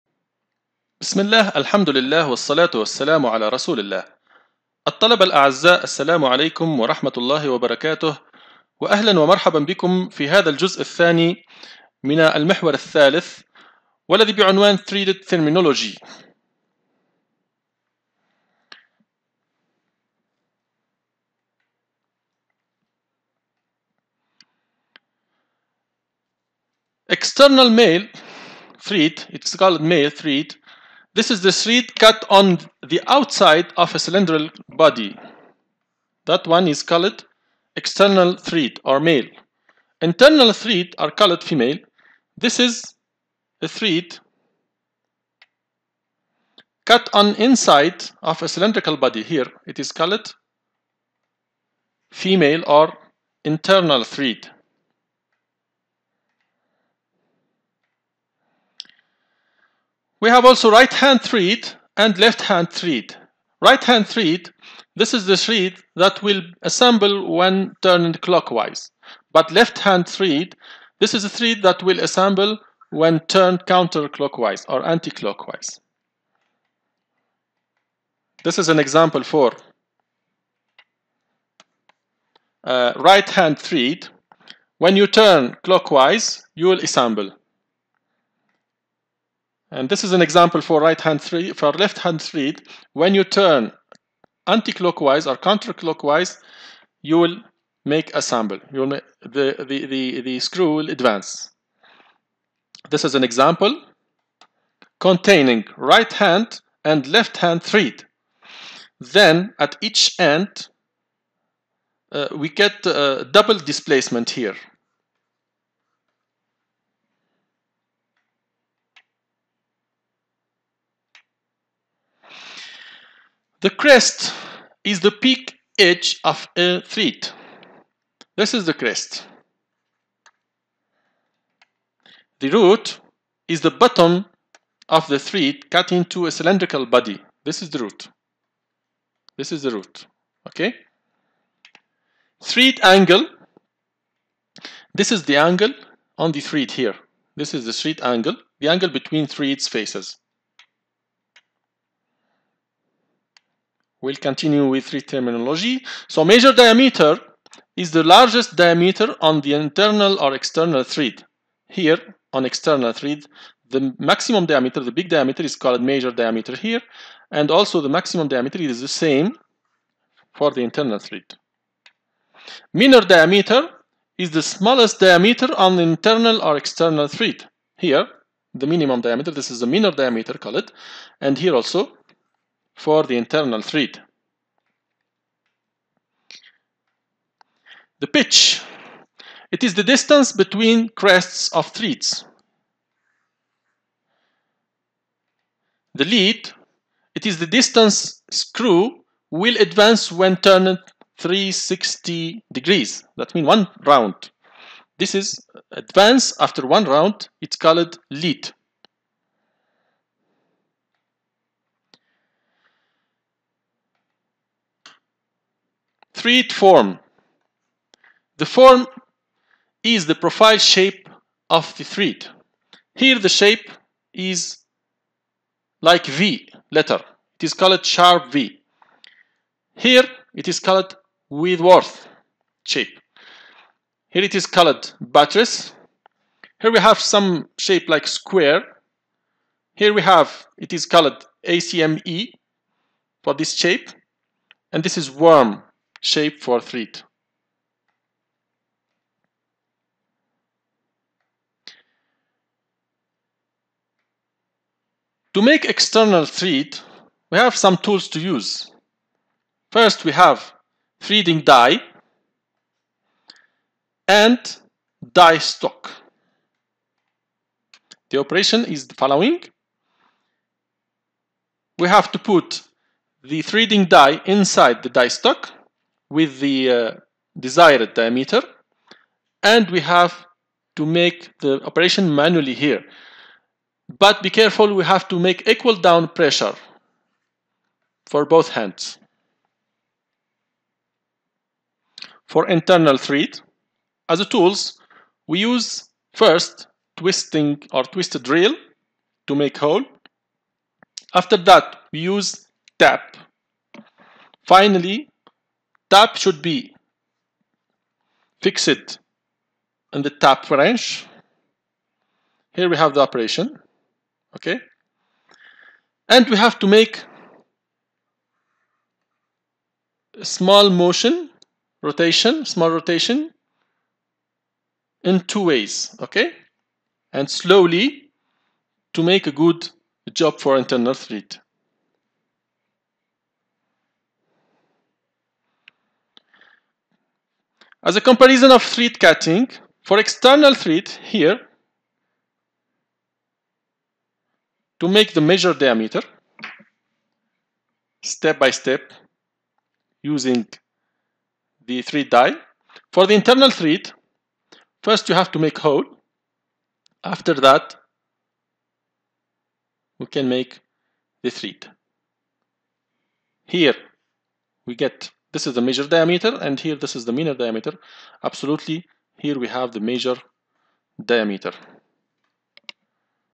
Engineering Drawing 2 | Ch3 | Thread terminology
المقررات المفتوحة - الرسم الهندسي 2 - Engineering Drawing
Chapter 3
Threaded Fasteners
Thread  terminology
External (male) thread
Internal (female) thread
Right-hand thread
Left-hand thread